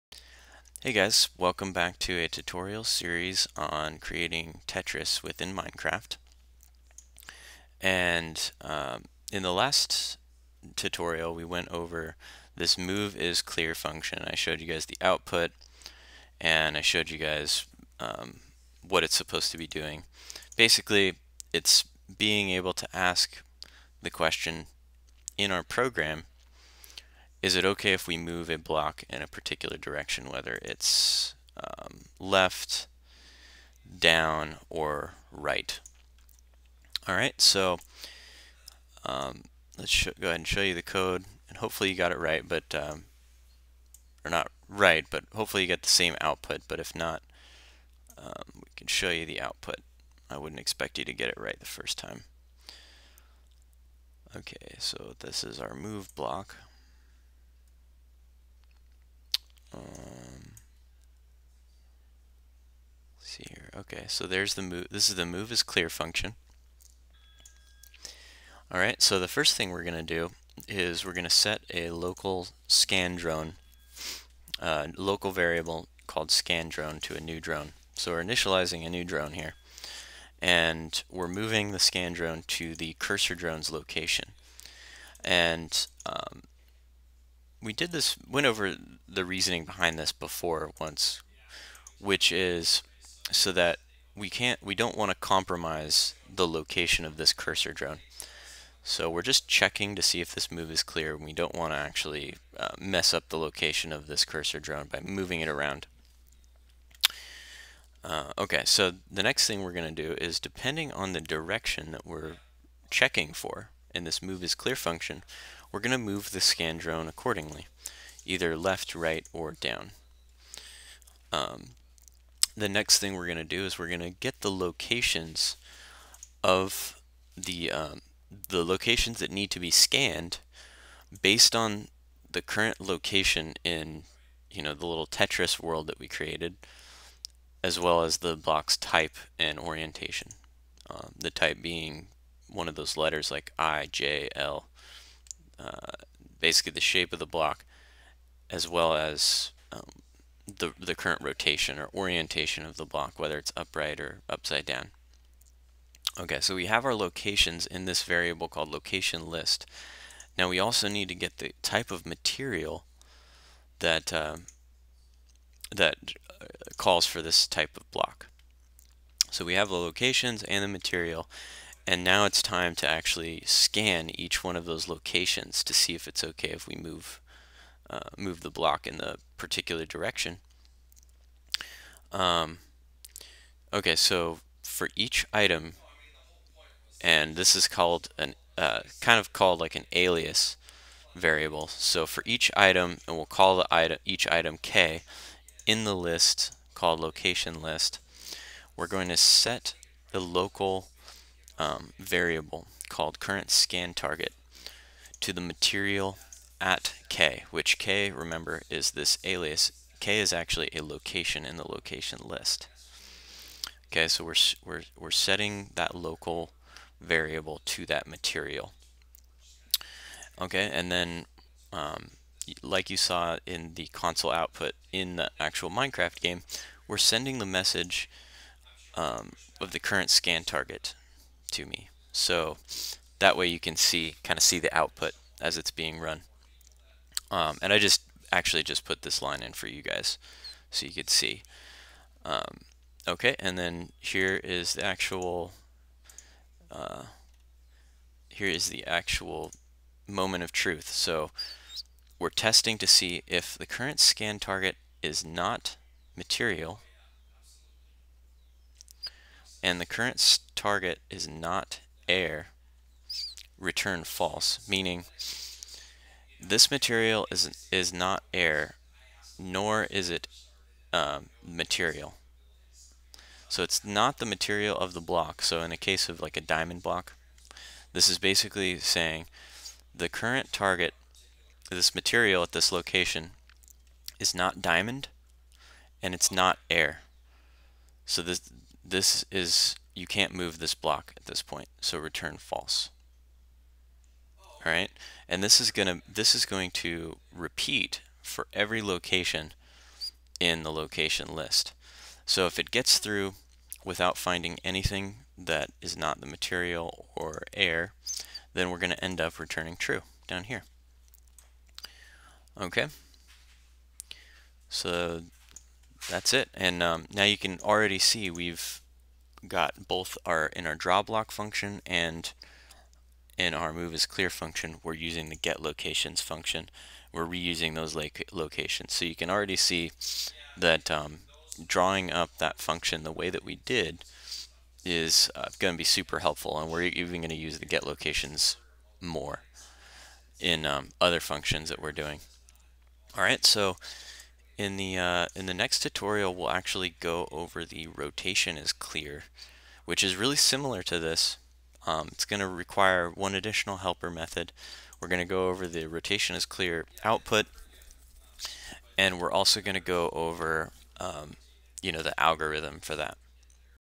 Game Logic - Tetris - 12 (moveIsClear Algorithm Code)
Learn how to create game logic such as game loops, and player controls, while implementing the game of Tetris within Minecraft.